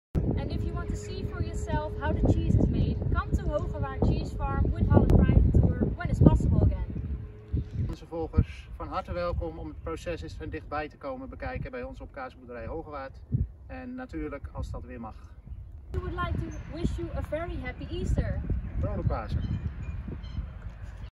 Visit Cheese Farm Hoogerwaard
Like to see where the Gouda-Cheester-Egg was made? You are very welcome to visit Cheese Farm Hoogerwaard with Holland Private Tour.